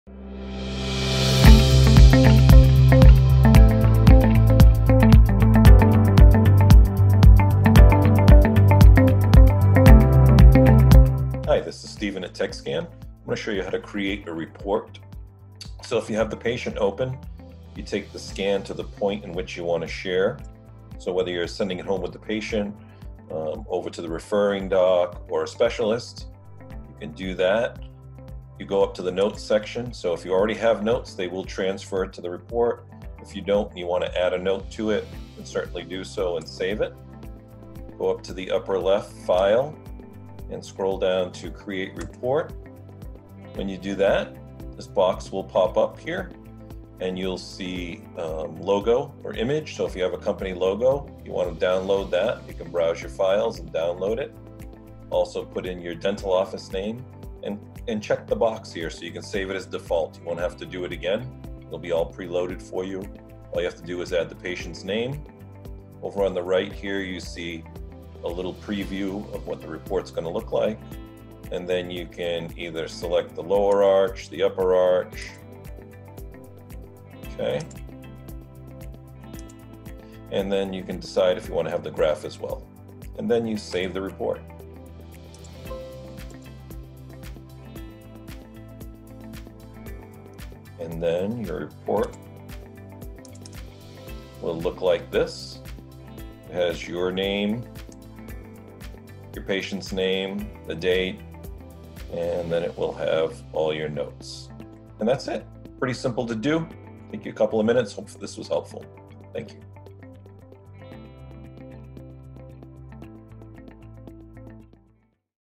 How to Create a Patient Report in T-Scan Software
In this video, Stephen Moniz, Regional T-Scan Sales Manager for Tekscan, takes you through the process of exporting patient reports in T-Scan Software. Patient reports are great for patients to pass along to doctors or specialists.